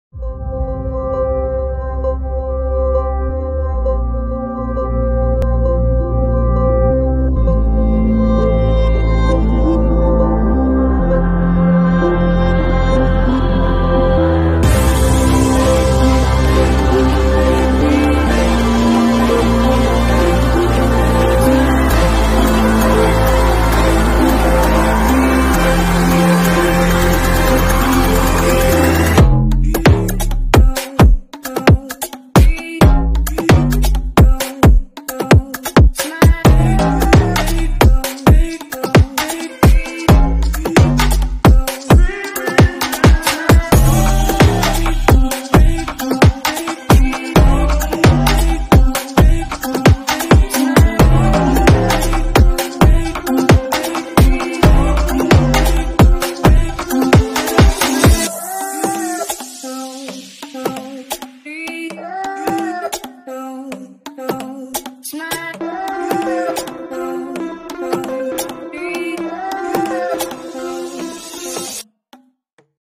$29 Amazon Titanium Wood Stove
First attempt cooking with this stove.
I love the smell of burning cedar.